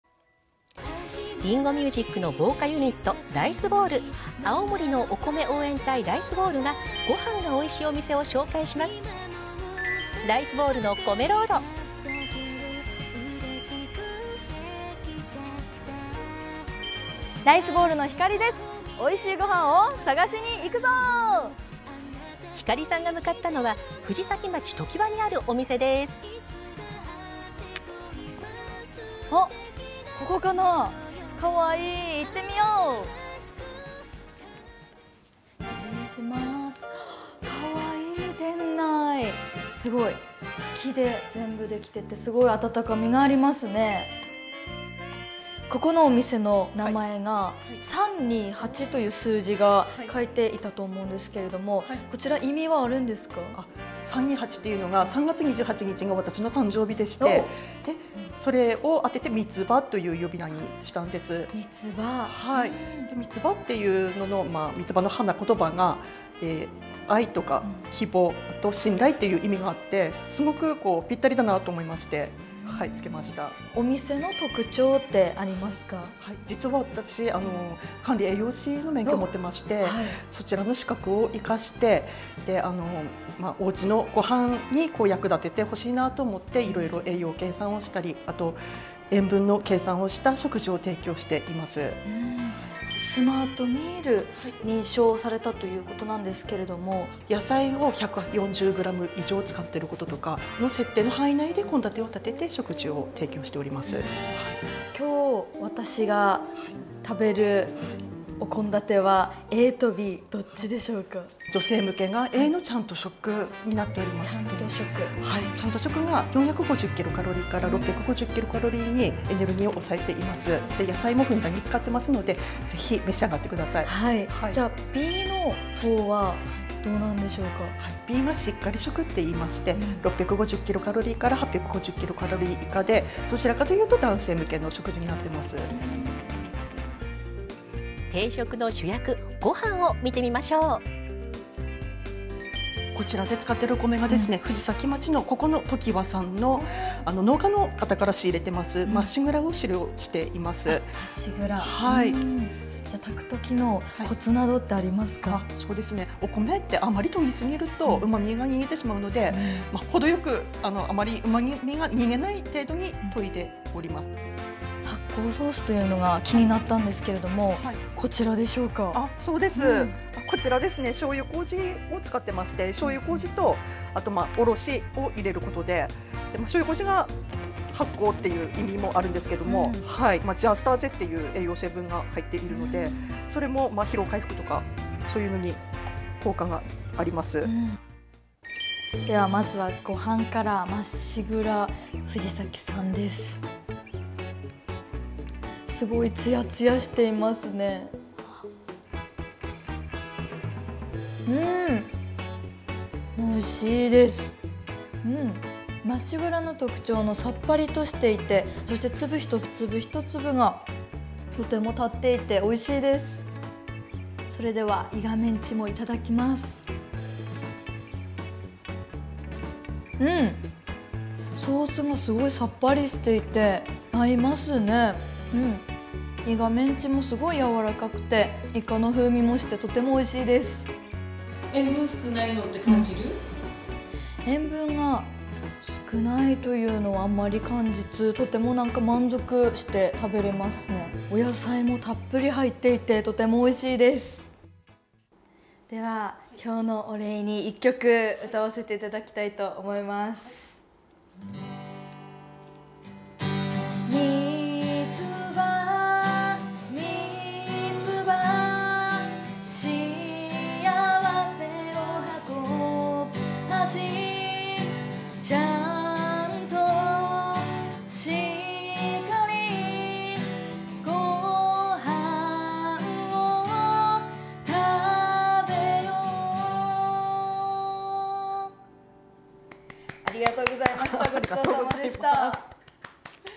ライスボールのこめロード9月4日管理栄養士さんのカフェ328
今回はライスボールの太陽が紹介します。藤崎町にある管理栄養士さんが経営するお店カフェ328。野菜がたっぷりでツヤツヤごはんのしっかり定食に太陽もにっこり！